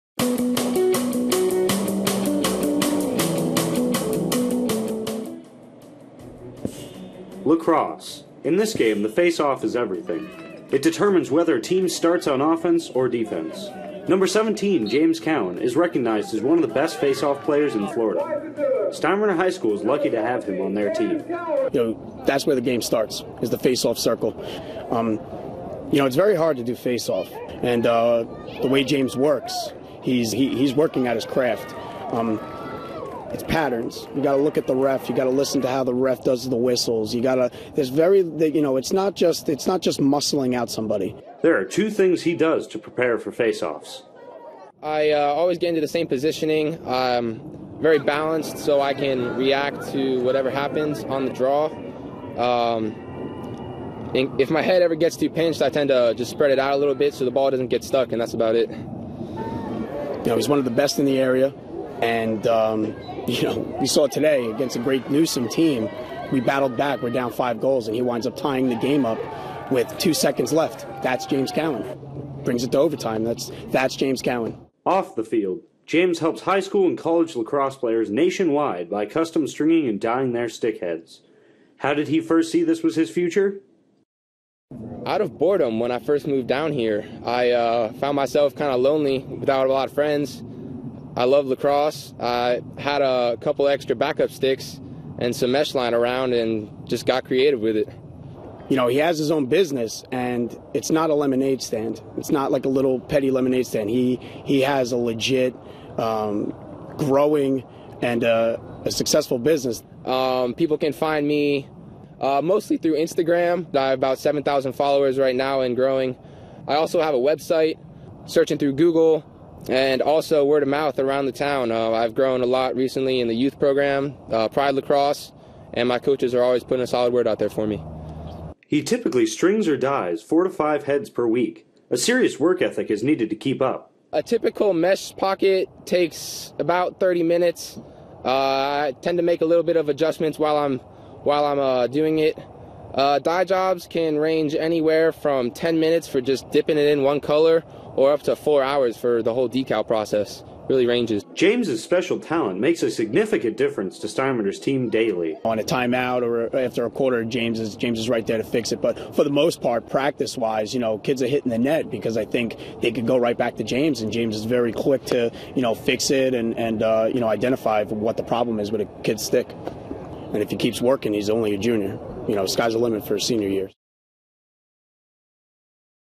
James Cowan Bright House Sports Network Feature
Here is a short film put together on me for a BHSN contest highlighting my faceoff X performance and my business off the field! My coach and I were interviewed after a 15-14 OT Loss to Newsome HS. There is also film from a few games of my season. All credit for the development and production of this video goes to BHSN Varsity Reporter Adler Shannon.

On another note, my junior year highlights will be up soon!